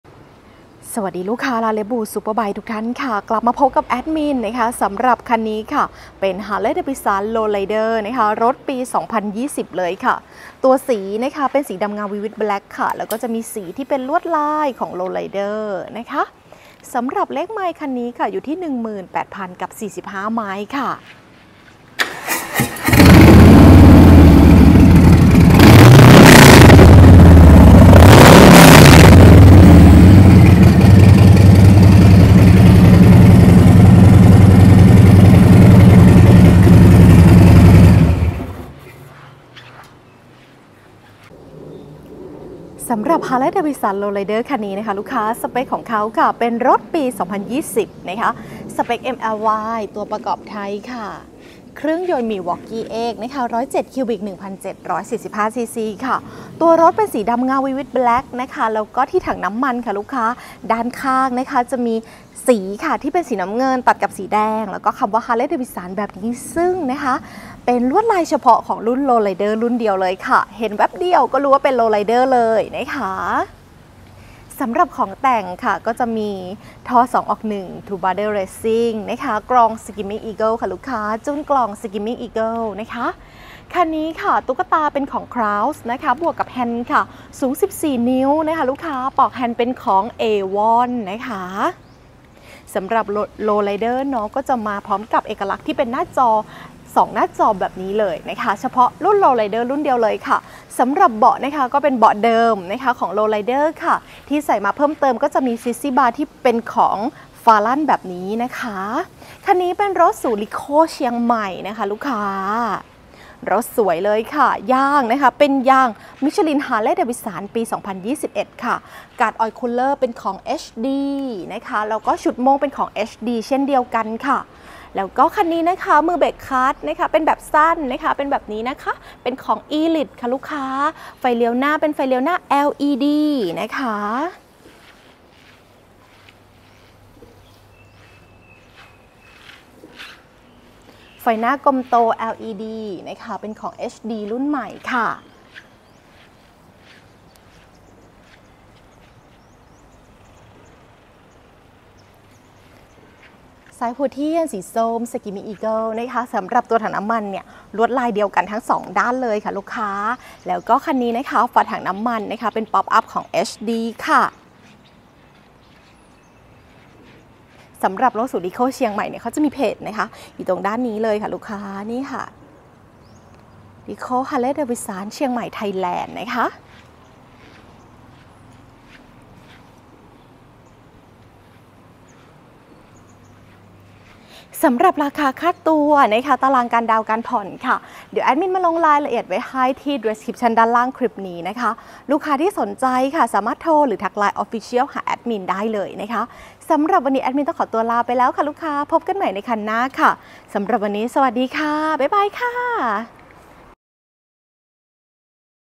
รีวิวฮาร์เล่ย์ HD FXLR LOW RIDER รถปี2020 เครื่องยนต์ 107CUBIC (สี Vivid Black) 🇺🇸⚡️
Harley Davidson FXLR LOW RIDER
รถปี : 2020
เลขไมล์ : 18,000 ไมล์
SPEC : MY
เครื่องยนต์ : 107Cubic (1745CC)
สี : Vivid Black 🇺🇸⚡️
: รายละเอียดของแต่ง :
- ท่อ TBR
- กรอง Screamin Eagle
- จูนกล่อง Screamin Eagle
- ตุ๊กตา KRAUS & แฮนด์ 14"
- มือเบรค&คลัทซ์ ELITE
- Sissy bar ⚡️
- ชุดโม่ง HD
- ไฟเลี้ยวหน้า LED
- สายหัวเทียน Screamin Eagle
- ฝาถังน้ำมัน Pop Up HD
- ปลอกแฮนด์ AVON
- กาด Oil Cooler HD

💰ขาย 758,000.-
สำหรับจัดไฟแนนซ์ ดาวน์ 118,000 + ค่าจัดไฟแนนซ์ 15,000
( รวมออกรถ 133,000 )
( ไม่รวมประกันชั้นหนึ่งนะคะ )
💸ผ่อน 16,549 / 48 งวด
💸ผ่อน 14,266 / 60 งวด

📲Line Official : @ravebullspb (มี@ด้วยนะคะ)
🖥️ เพจร้าน : Rave Bulls Superbike